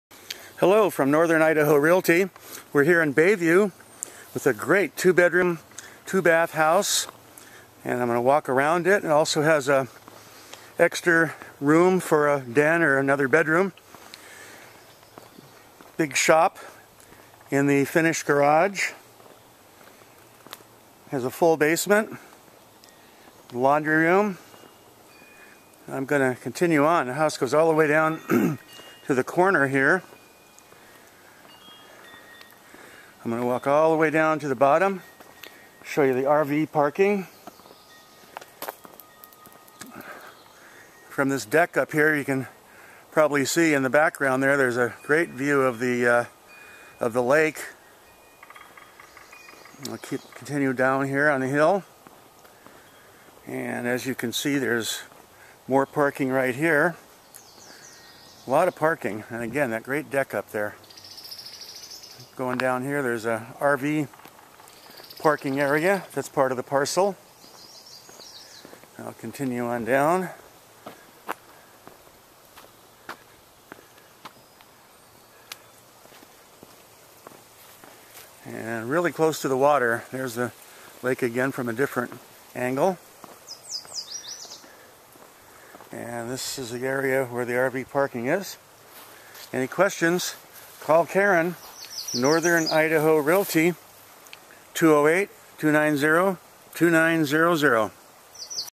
Bayview Idaho
Great home in Bayview Idaho. Really close to the lake with beautiful water views. Big lot with RV parking. call Karen at Northern Idaho Realty for more info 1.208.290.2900